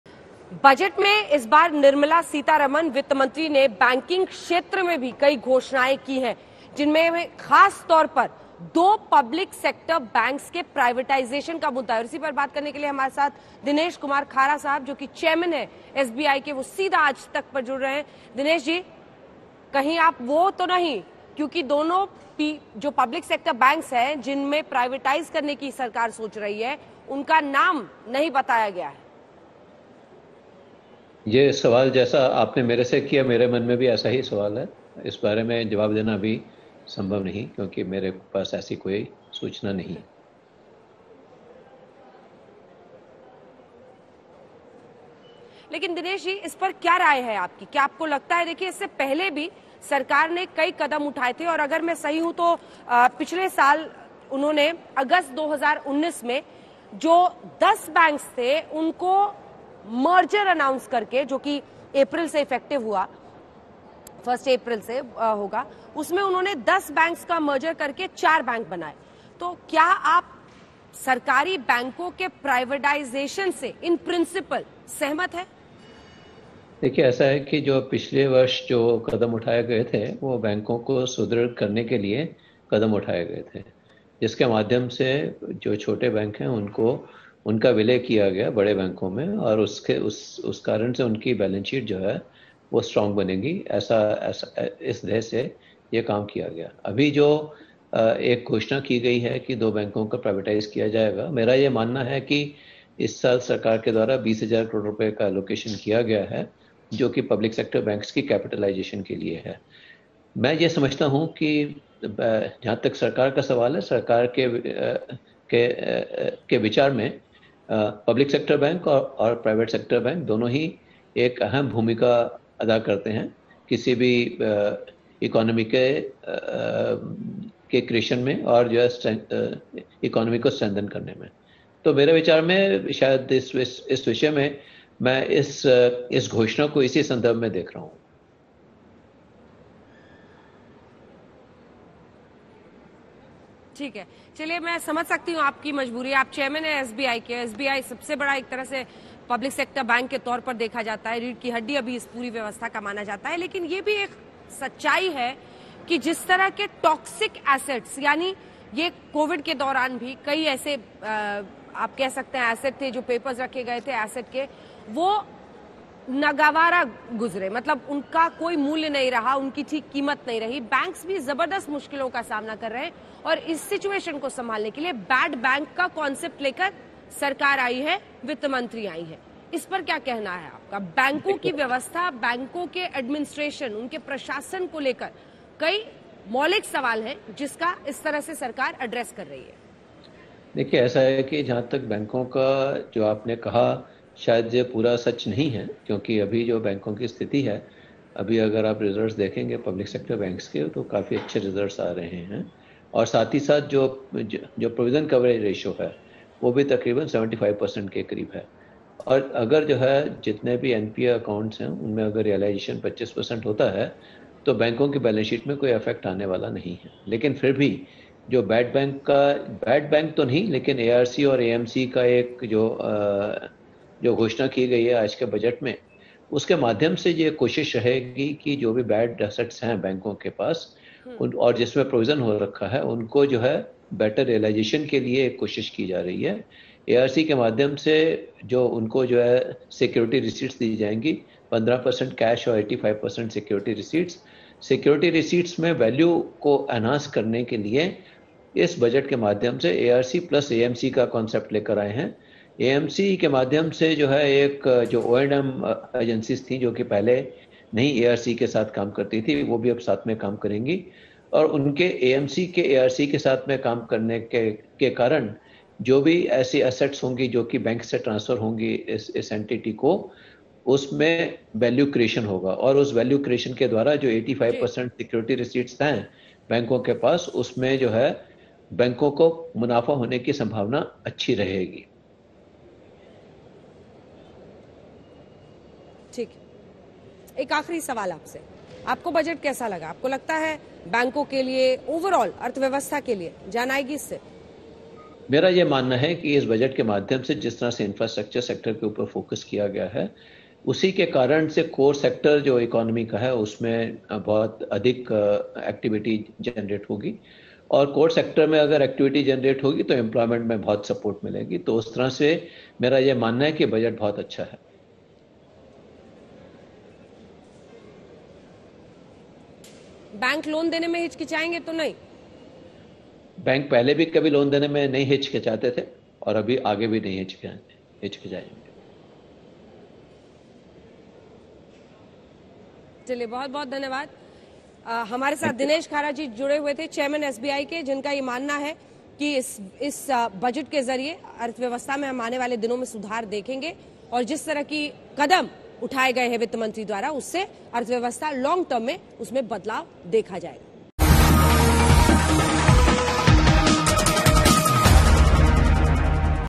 Budget 2021: बैंकों के प्राइवेटाइजेशन से कितना सहमत? ये बोले SBI चेयरमैन
एसबीआई के चेयरमैन दिनेश कुमार खारा ने आजतक से बातचीत में कहा कि बजट बहुत अच्छा इससे रोजगार बढ़ेंगे.